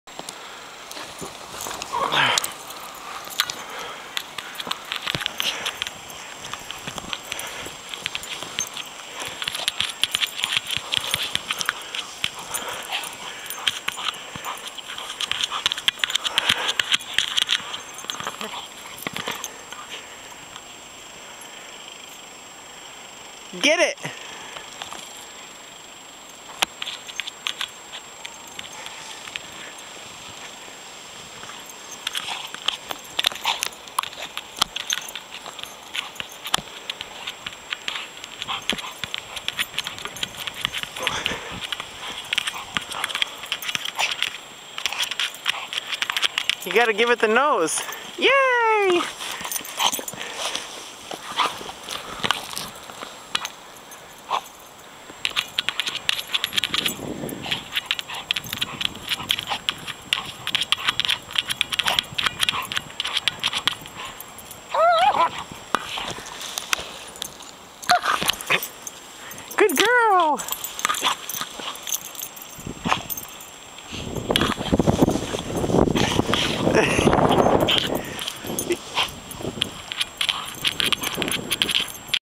Kola my German shepherd, playing with a ball in the snow
This is my German shepherd Kola playing with her ball after a big snowstorm.  She is 10 months old here.  Mano Vokieciu avigane zaidzia su sniegu.